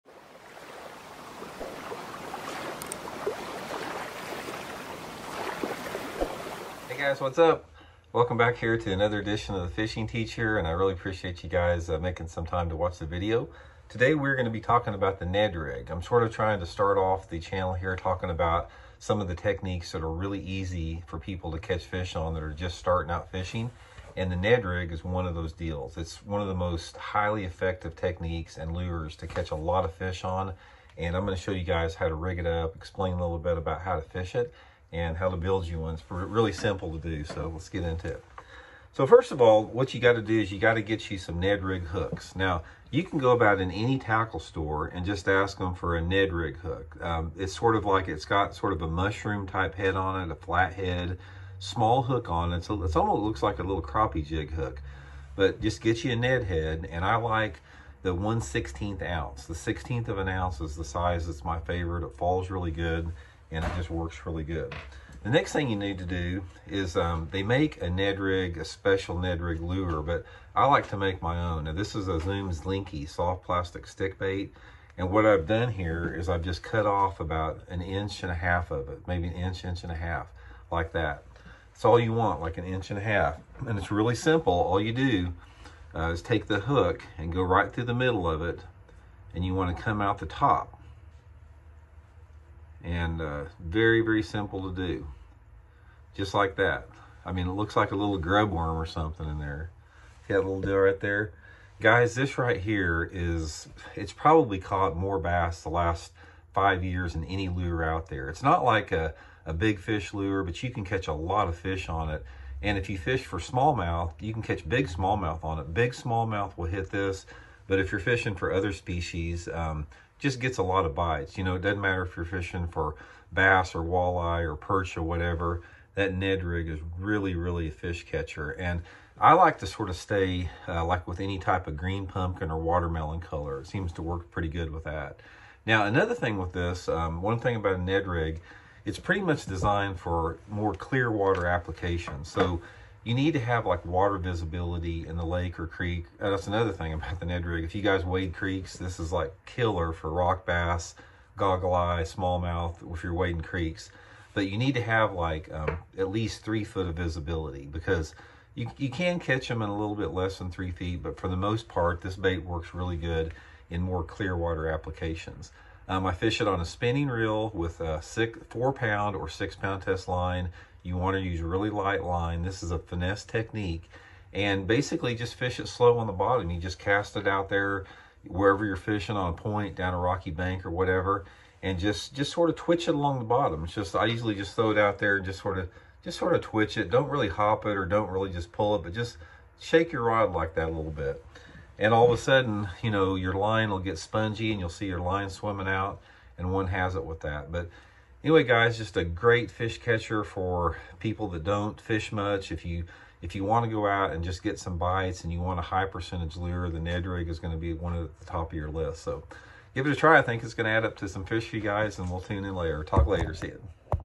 How To Rig And Fish A Ned Rig
Randy gives tips and advice on fishing a Ned rig…nedrig